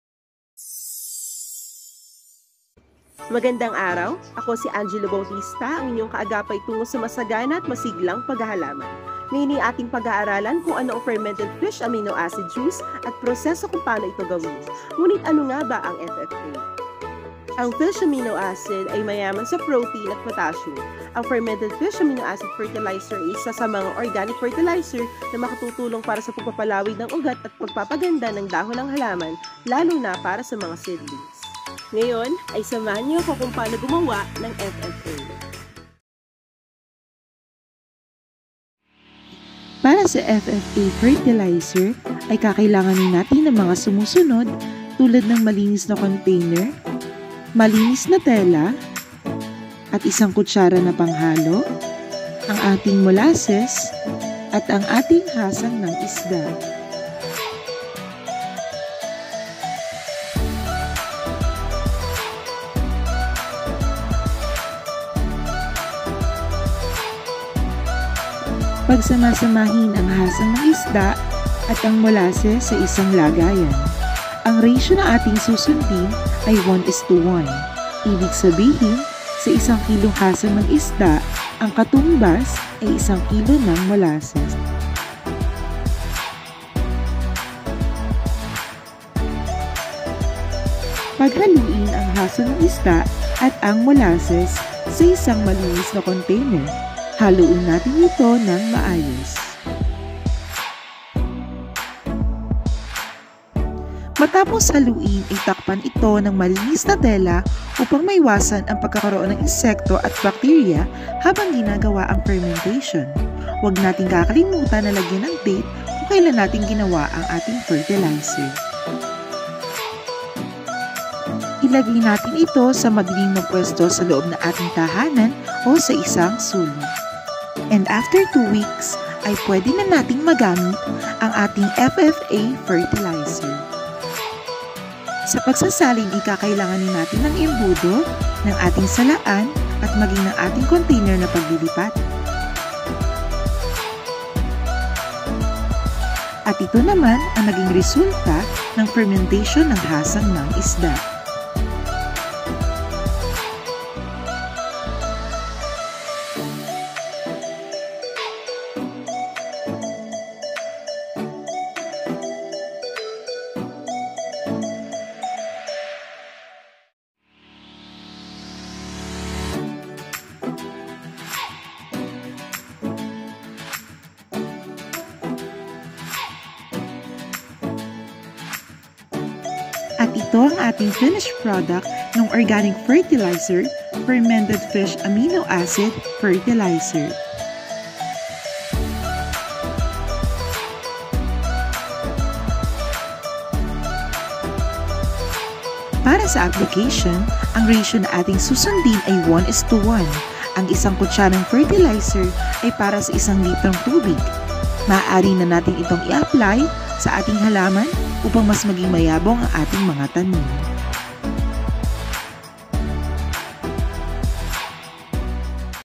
How to make a Fermented Fish Amino Acid Juice? | Organique Fertilizer by Angelo Bautista
Halina at matuto ng mga paraan para masaya at masiglang paghahalaman! Matuto ng mga panibagong kaalaman at kumita sa simpleng paraan.

Agri-Fishery